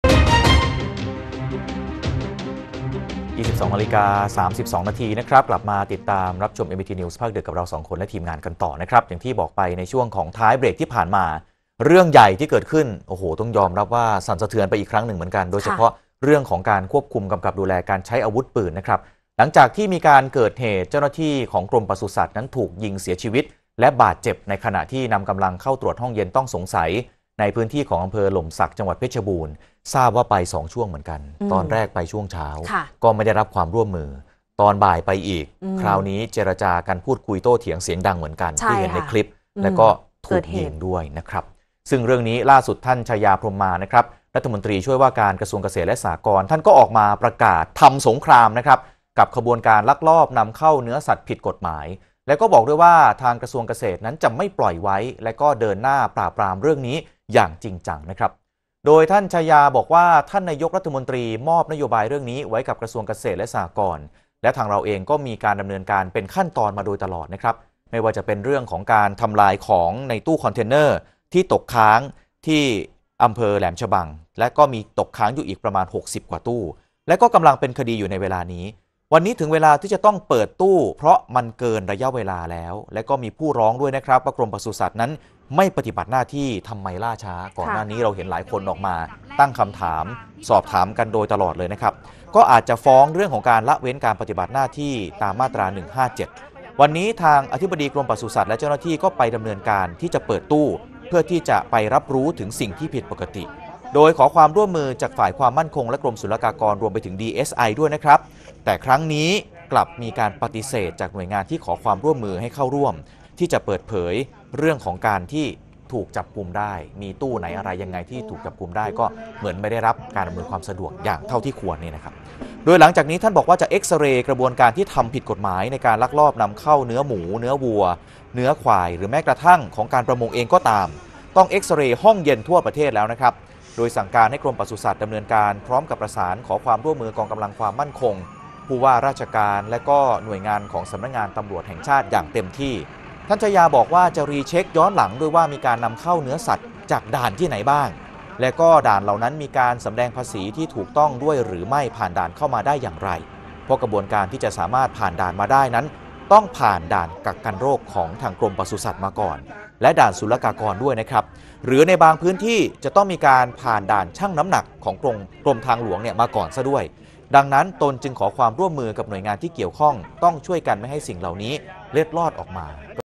ก.เกษตรฯ ประกาศคุมเข้มปราบนำเข้าเนื้อสัตว์เถื่อน ข่าวดึก วันที่ 12 ตุลาคม 2566
หลังเกิดเหตุเจ้าหน้าที่ปศุสัตว์ถูกยิงเสียชีวิตและบาดเจ็บ ขณะนำกำลังเข้าตรวจห้องเย็นต้องสงสัยในพื้นที่ อ. หล่มสัก จ. เพชรบูรณ์  ล่าสุด ท่านไชยา พรหมา รัฐมนตรีช่วยว่าการกระทรวงเกษตรและสหกรณ์ ก็ออกมาประกาศทำสงครามกับขนวนการลักลอบนำเข้าเนื้อสัตว์ผิดกฎหมาย พร้อมระบุ ก.เกษตรฯ จะไม่ปล่อยไว้ และเดินหน้าปราบจริงจัง